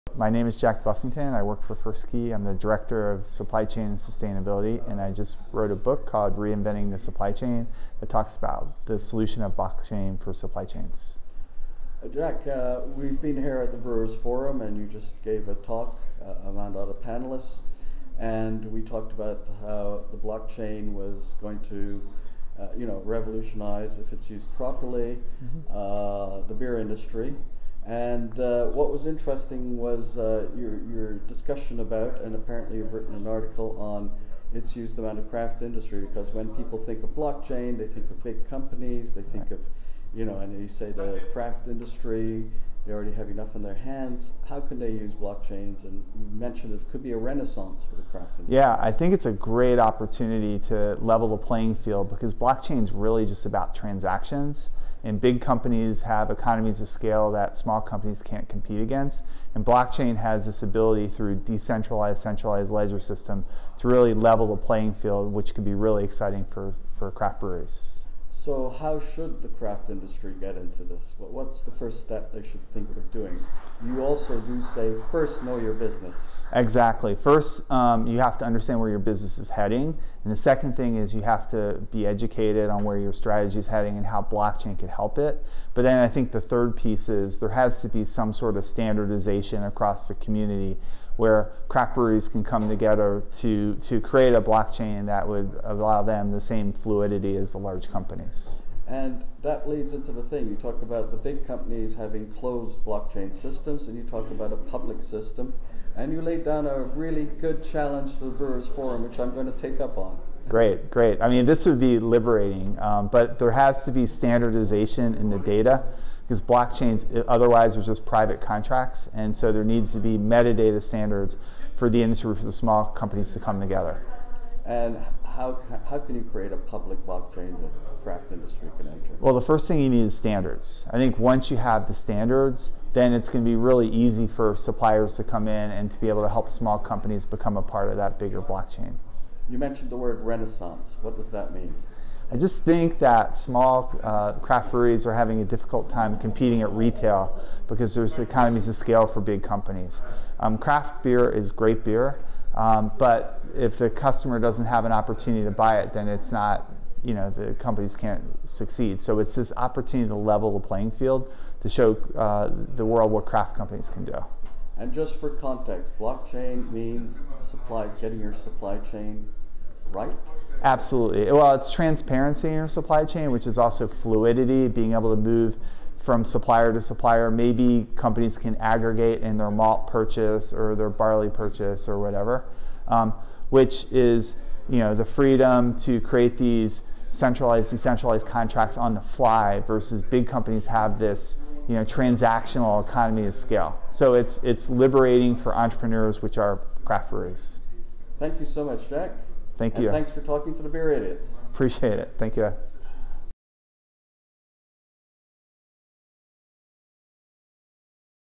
Jack Buffington: blockchain for small brewers
Jack Buffington, director of Supply Chain & Sustainability, talks to the Beer Idiots about his new book ‘Reinventing The Supply Chain: A 21st-Century Covenant With America’. In the book he discusses the potential of blockchain, part of his talk at the Brewers of Europe forum in Prague about how small brewers can leverage the technology along the supply chain.